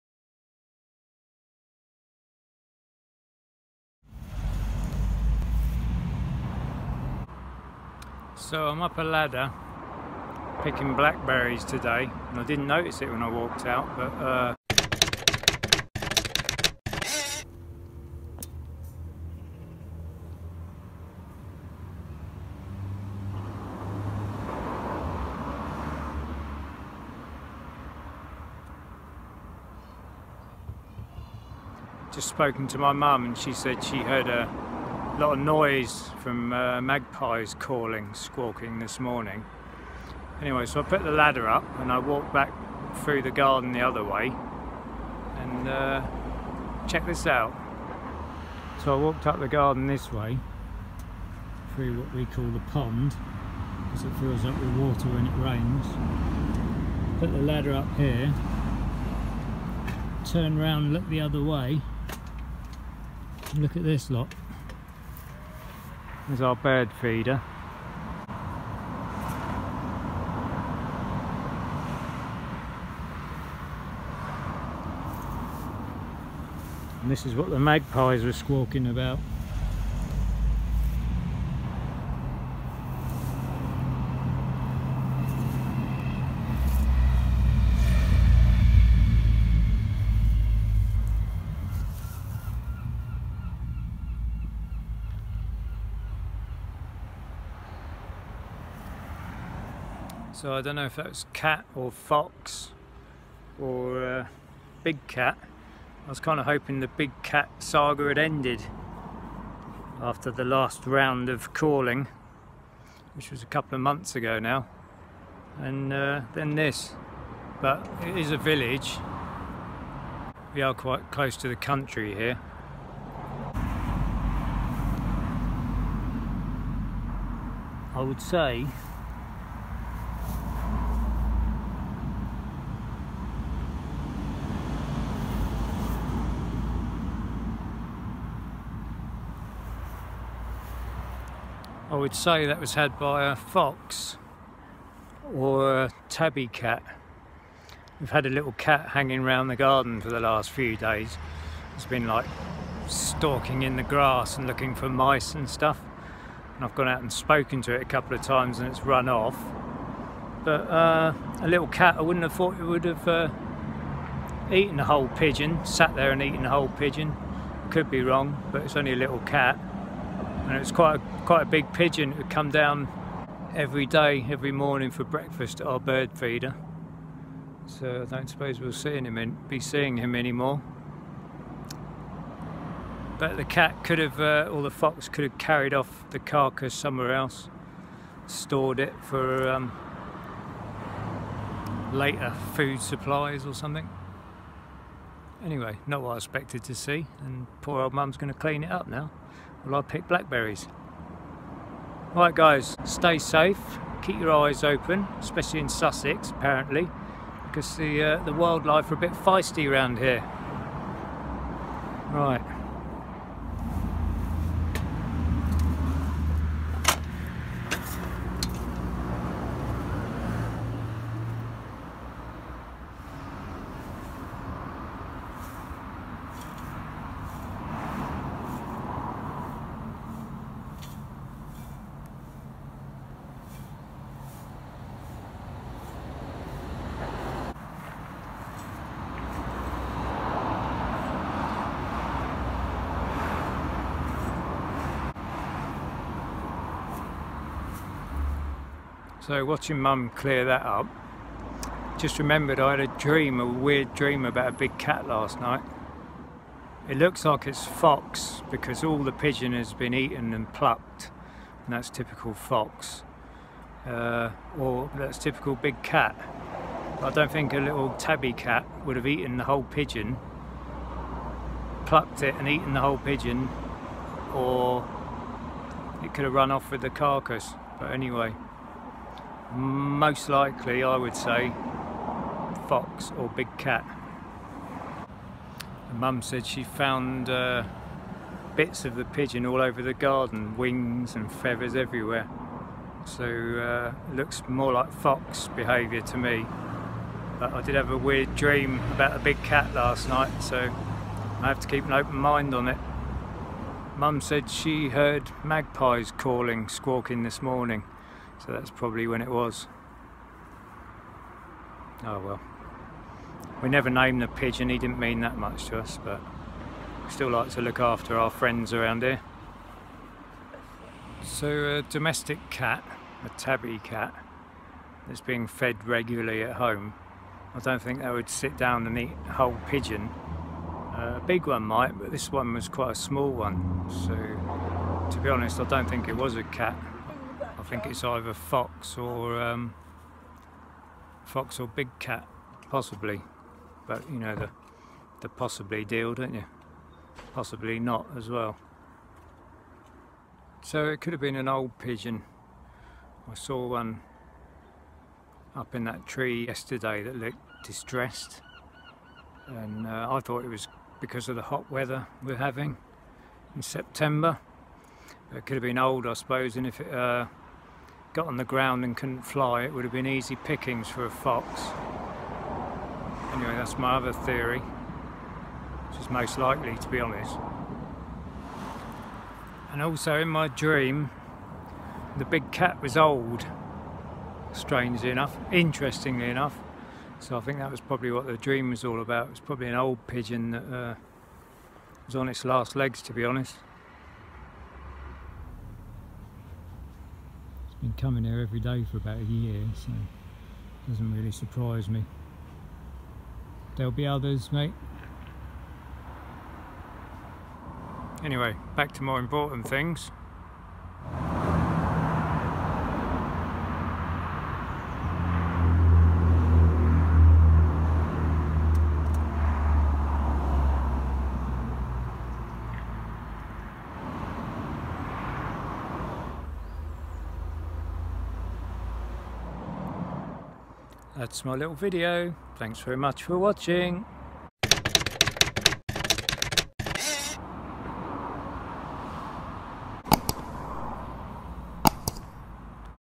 BIG CAT, Fox, Domestic Tabby or.... Possibly? - Part 6 - Big Cats in the U.K.
I found a pigeon carcass in the garden. what do you think dod this?